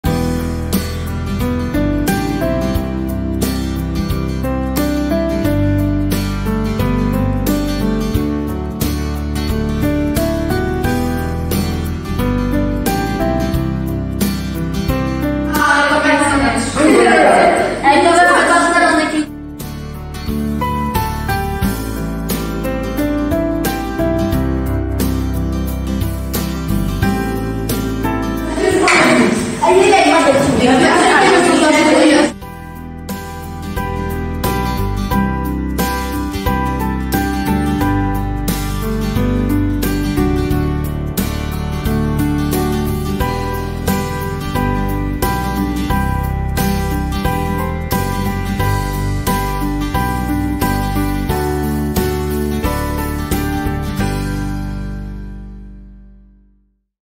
Erasmus + Wieże SP Szarów
Kolejne Spotkanie  Erasmus + Wieże-
upowszechnianie wiedzy o wspólnym europejskim dziedzictwie i różnorodności w Szkole Podstawowej w Szarowie.
Przygotowania do mobilności edukacyjnej.